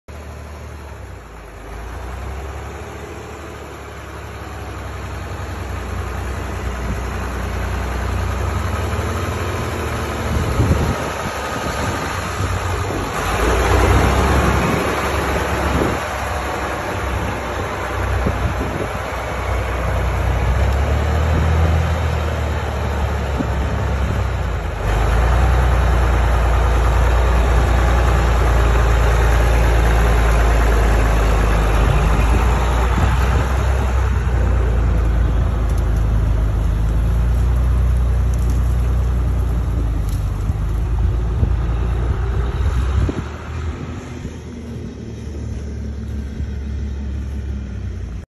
OLIVER 1750 For Sale
1750 Oliver 80 horse power diesel 1 remote 540 Pto power steering is week runs and gears out the best